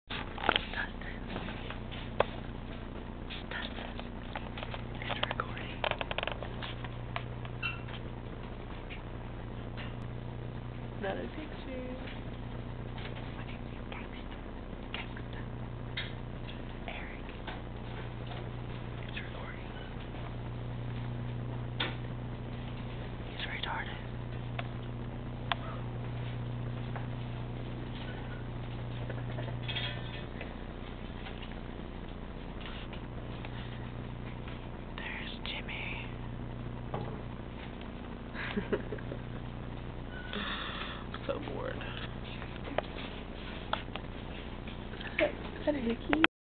Government class [SO BORING]
The most boring class taht takes place in my life...But i try to enjoy it.